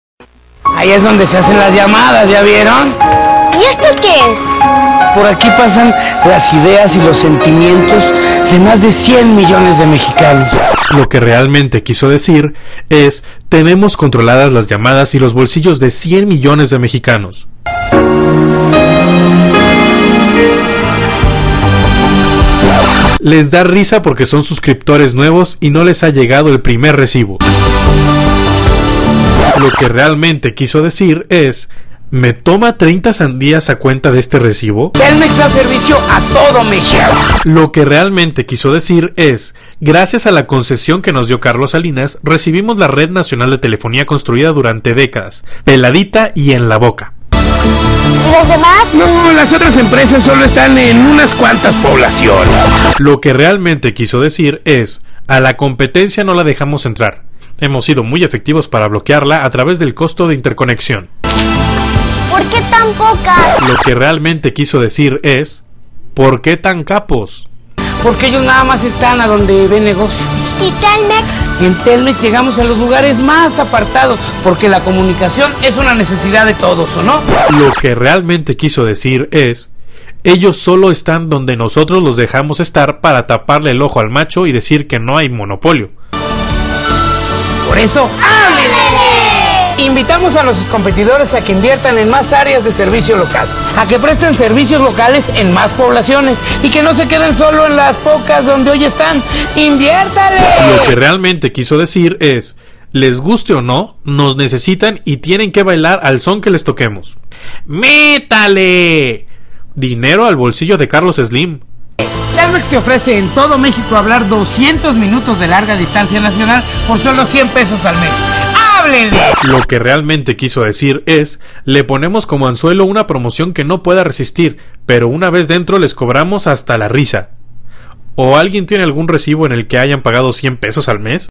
Entiéndale
La cruel verdad sobre Telmex y sus intenciones.( o que; a poco me van a decir que los de Telmex son unas blancas palomas)